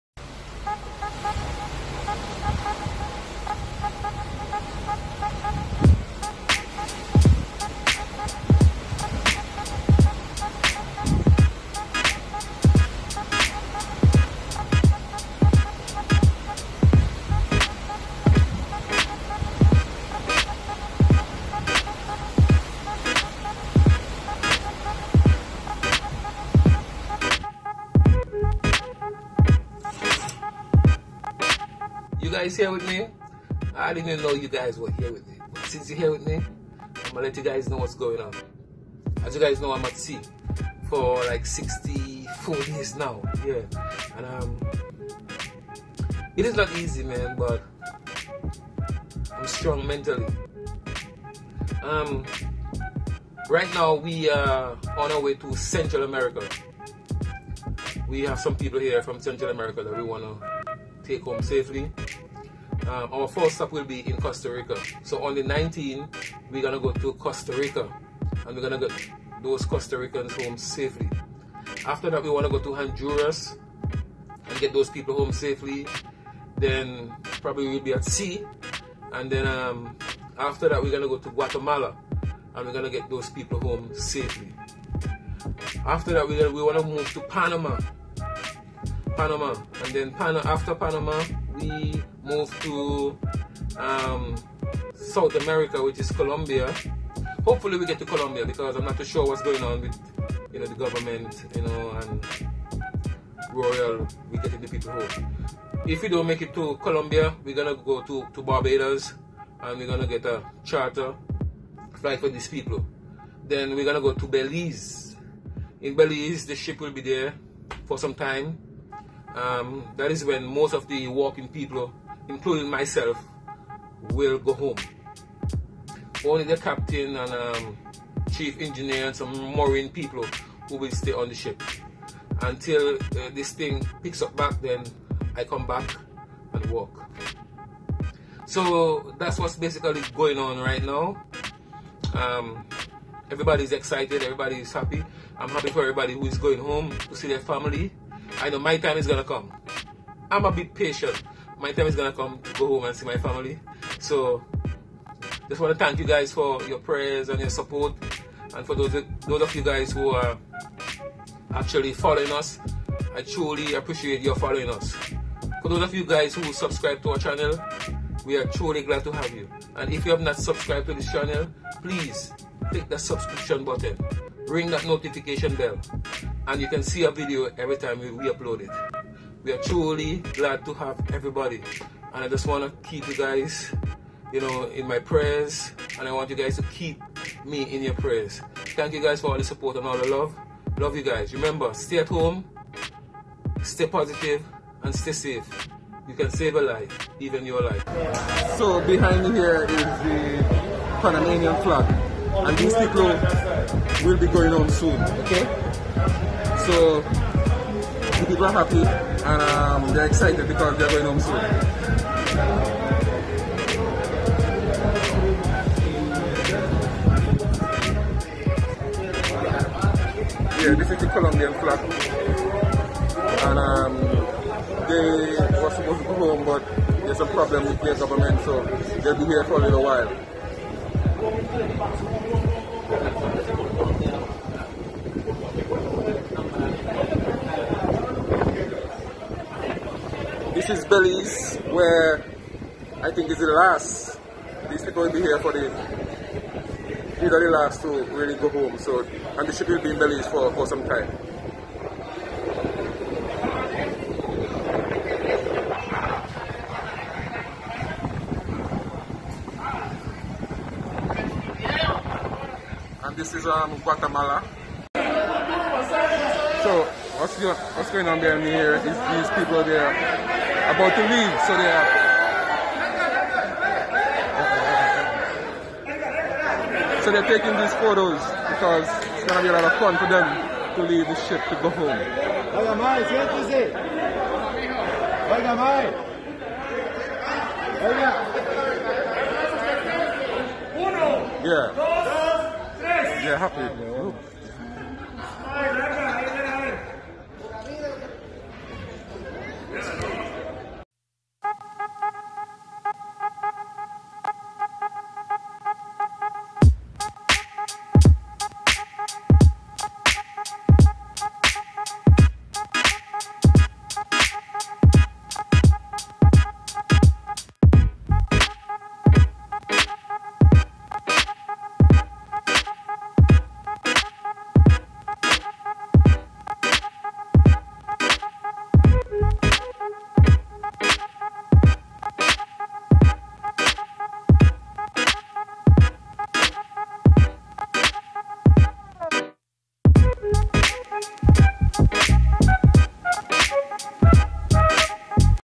VLOG: I'm Ready to go home!| Royal Caribbean|COVID-19 Lockdown
Welcome EVERYONE, we hope you are all well. The repatriation process is finally here. After transporting crew from different parts of the world to other ships, the Rhapsody will be taking home crew from Central America, South America, and the Caribbean. Here's a short video with updates. Please continue to keep us in your thoughts and prayers.

Remember to:
Like 👍before you LOOK
SHARE 📲 if you CARE ☺️